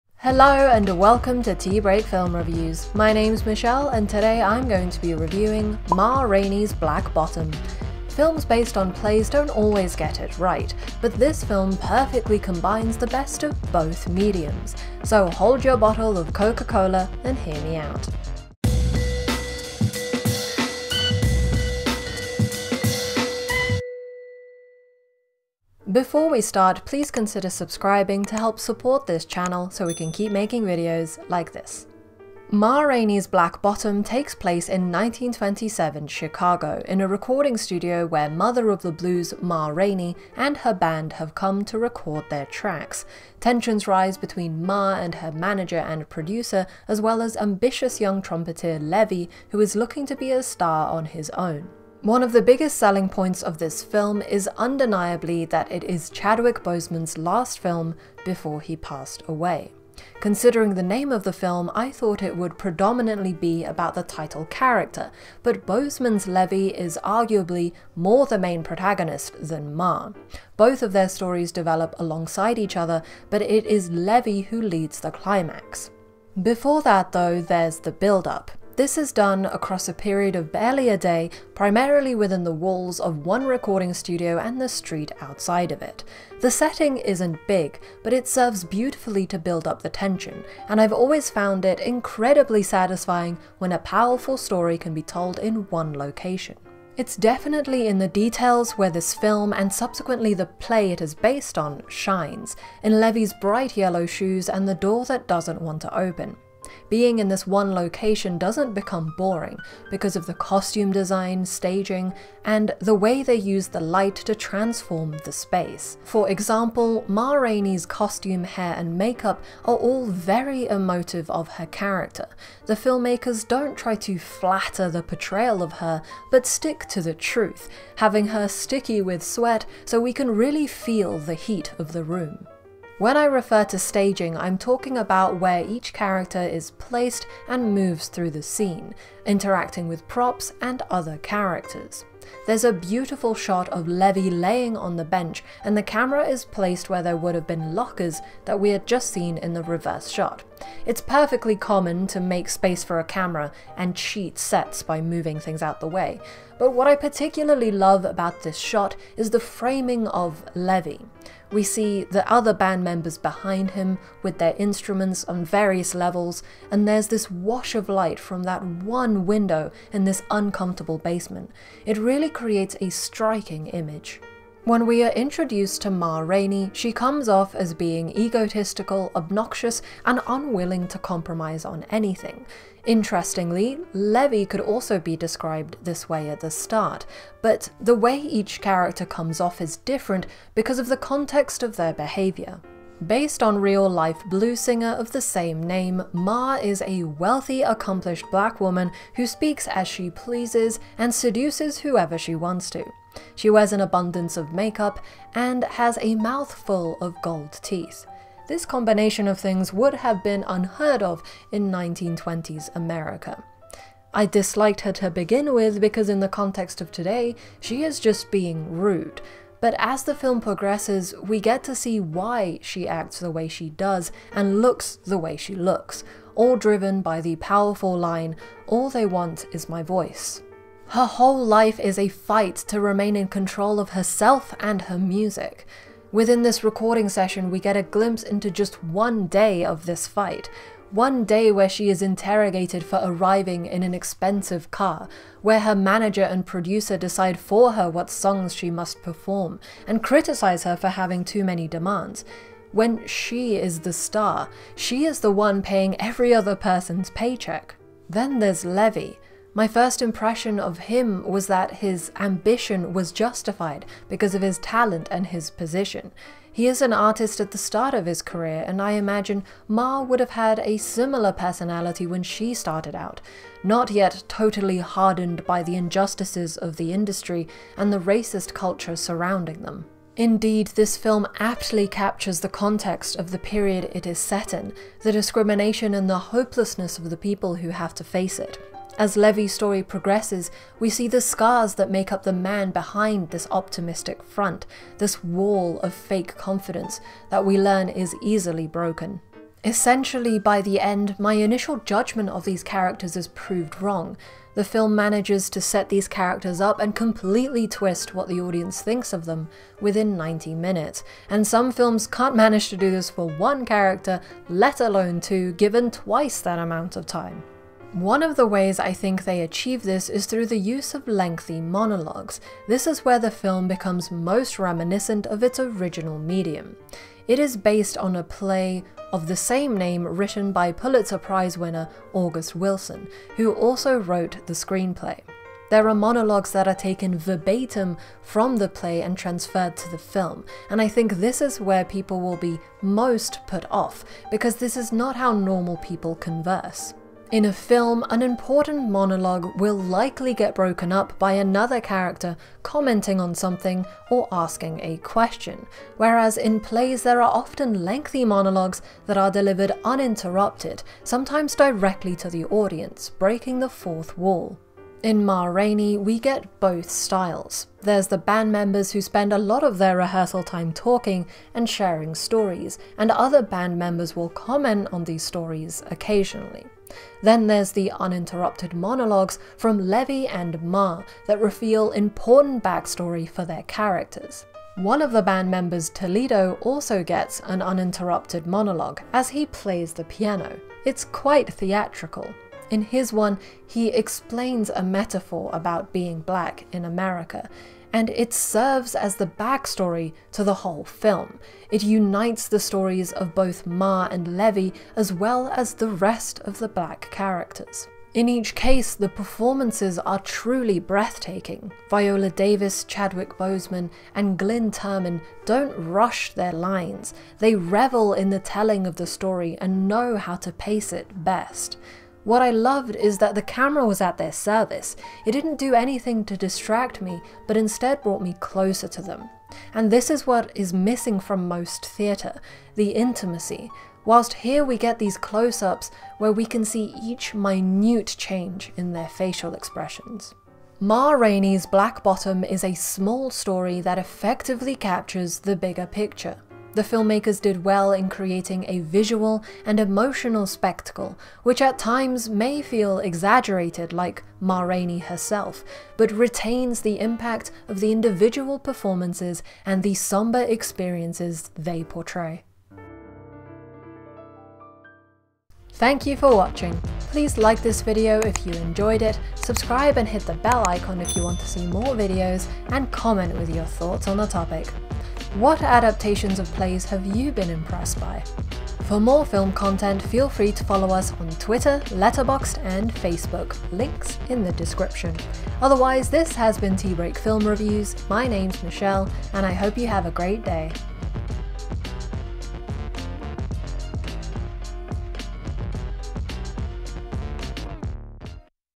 Chadwick Boseman shines! | Ma Rainey's Black Bottom (2020) | Film Review | TBFR
Michelle reviews Ma Rainey's Black Bottom (2020) Directed by George C. Wolfe, starring Viola Davis and Chadwick Boseman.

Filmography
- Ma Rainey’s Black Bottom (2020) Directed by George C. Wolfe, USA: Netflix.

Bibliography
- Wilson, August (1982) Ma Rainey’s Black Bottom, USA: Plume